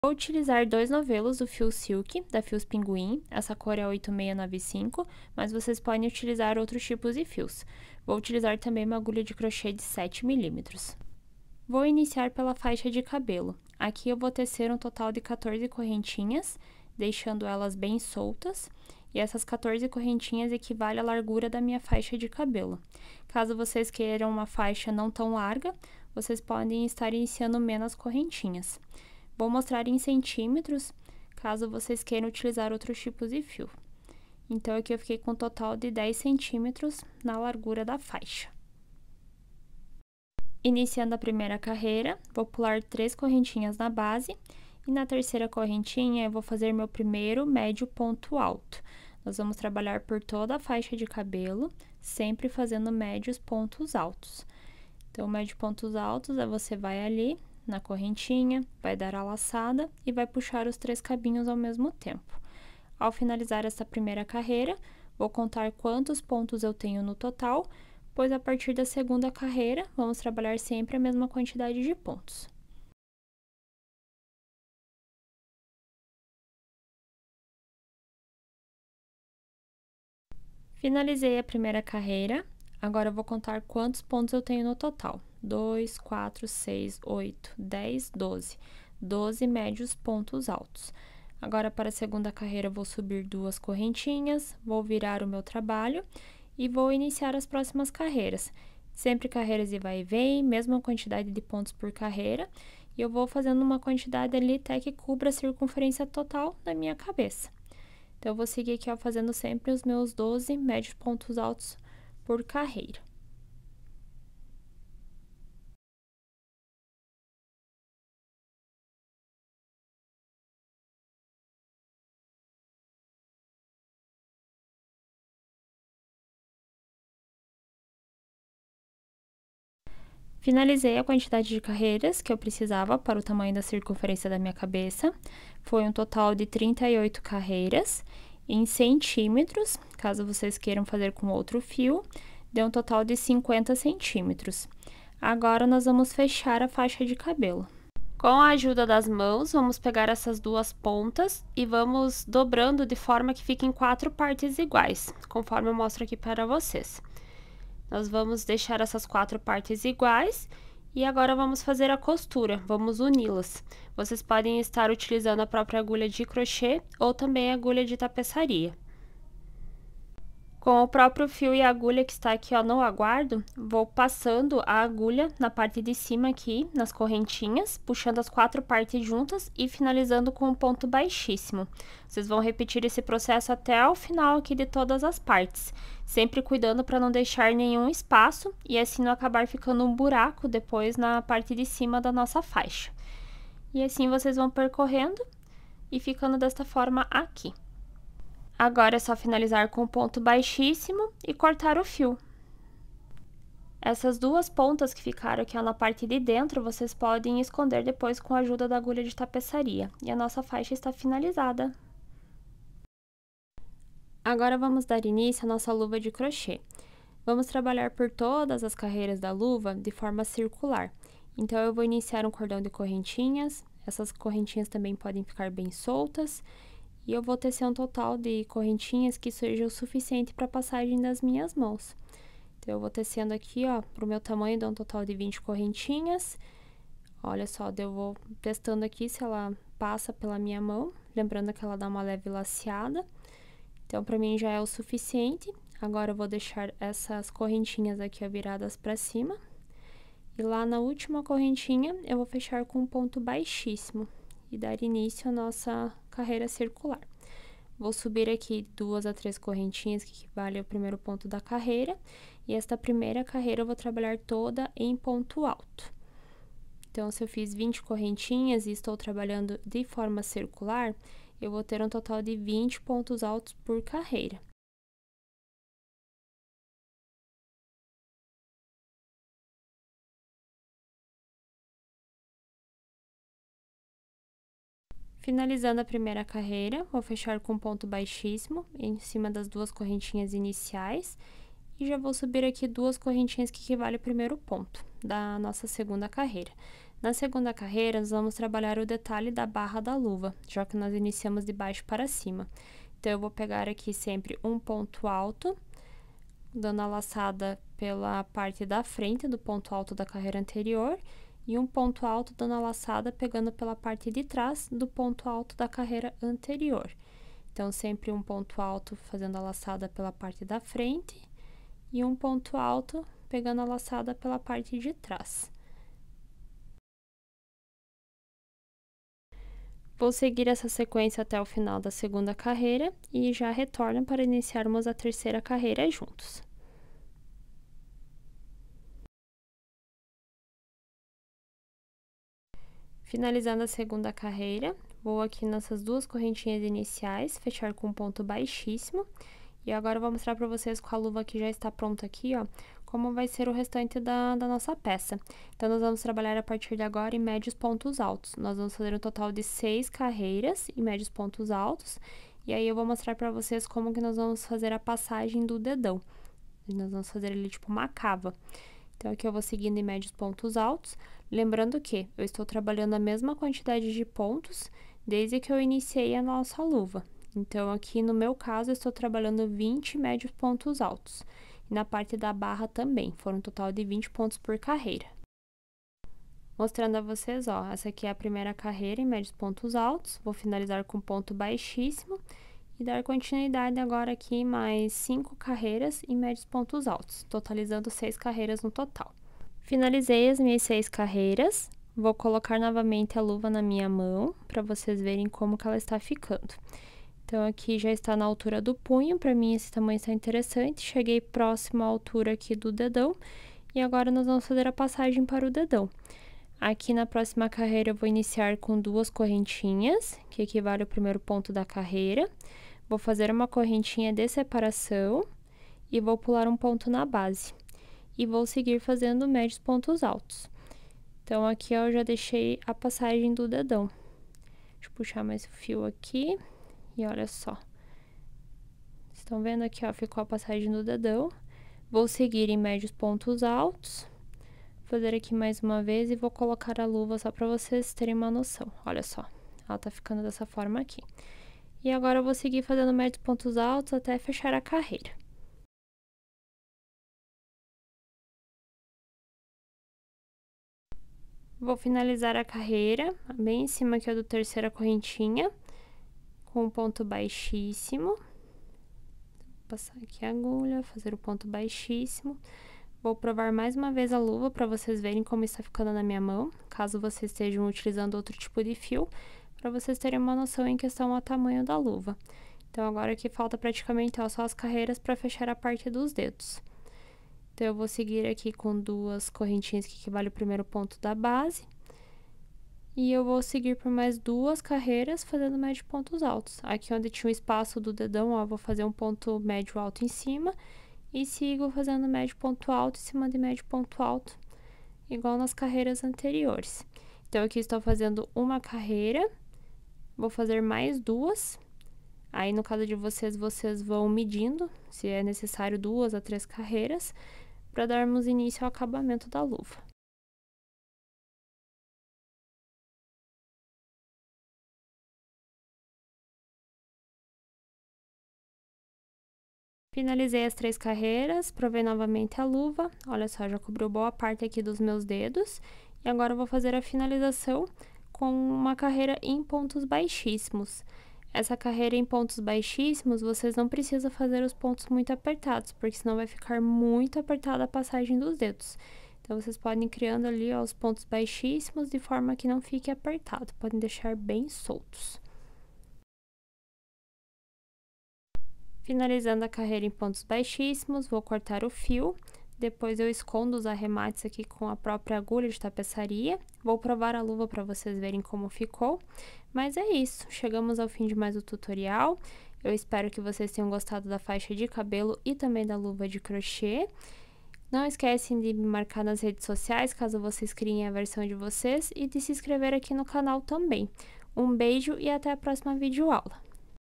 Faixa de cabelo e luvas de crochê - Headband crochet
Faixa de cabelo e luva de crochê

 2 novelos Silky (fios Pingouin)
Mas como ensino sob medida é possível adaptar com outros tipos de fios facilmente,

Agulha de crochê n°7mm ou de sua preferência

Agulha de tapeçaria e tesoura